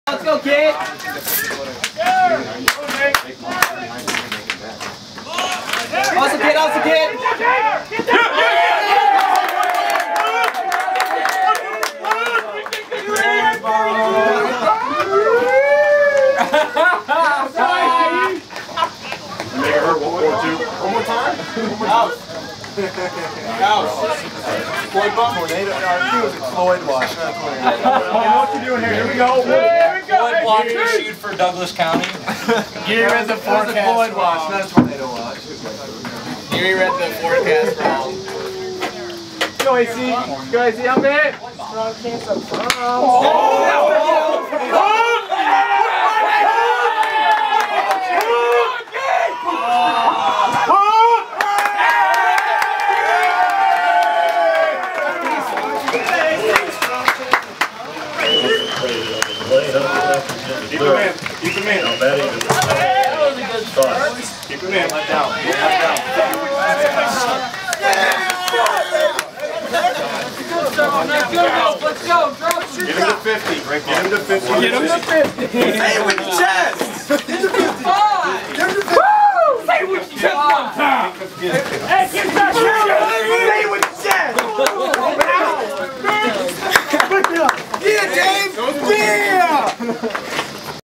Duesning 52's Districts at Millard South HS Omaha NE
Duesning 52's dugout after rain delay playing against SKUTT at Millard South High School, Omaha NE July 21, 2013